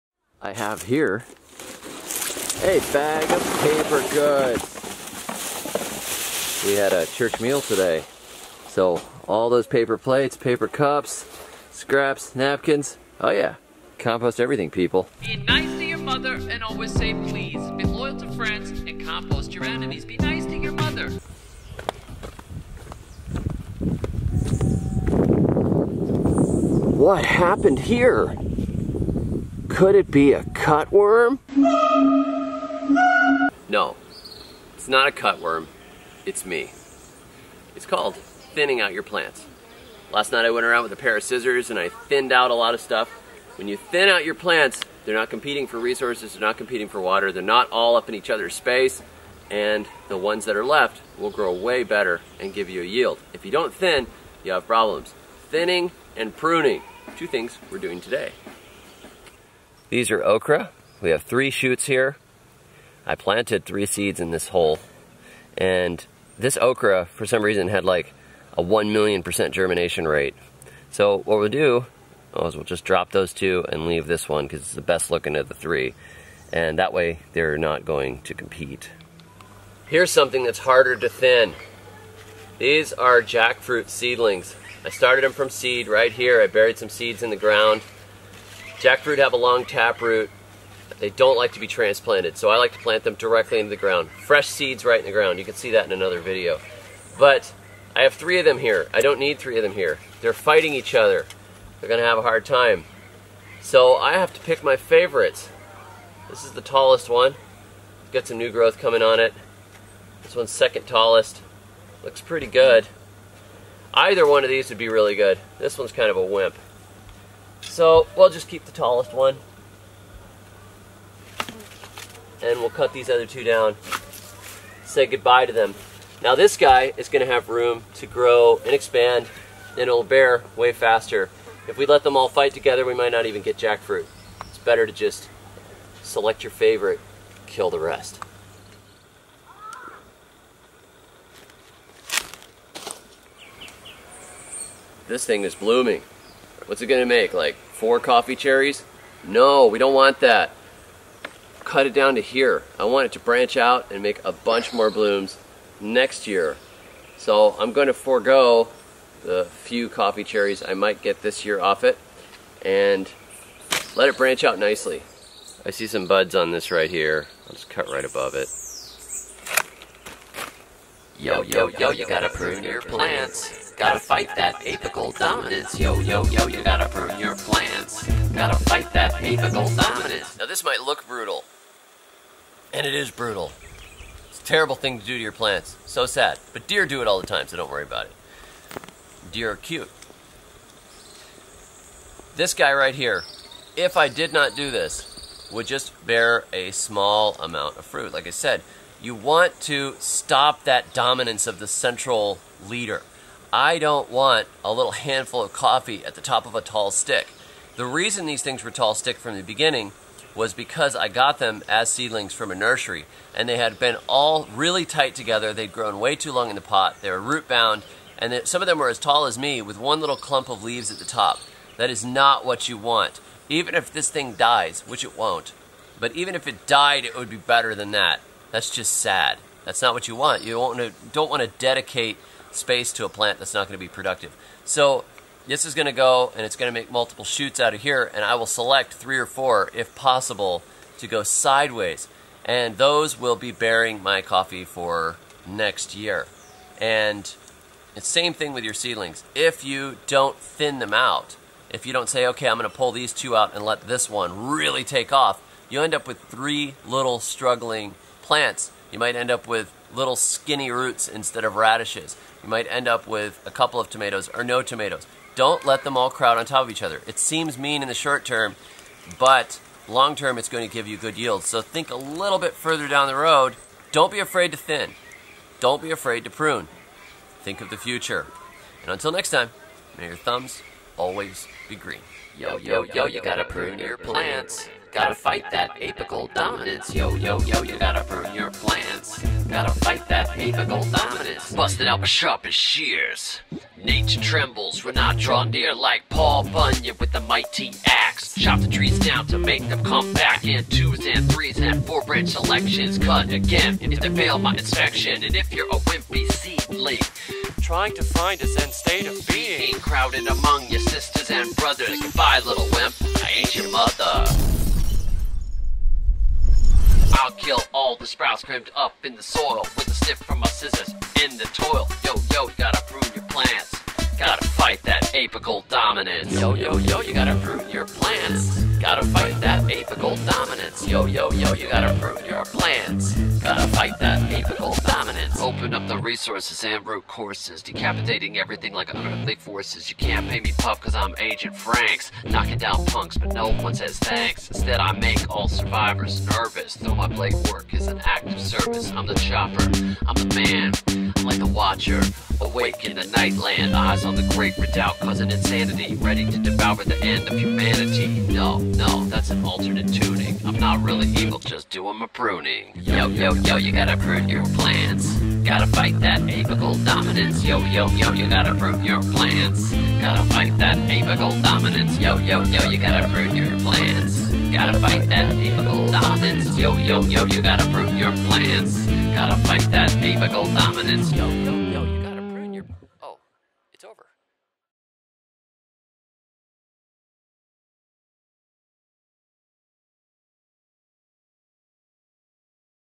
Thin and Prune With Total Brutality
Don't be weak. Prune your plants. Thin your seedlings. THEN RAP ABOUT IT.

David's books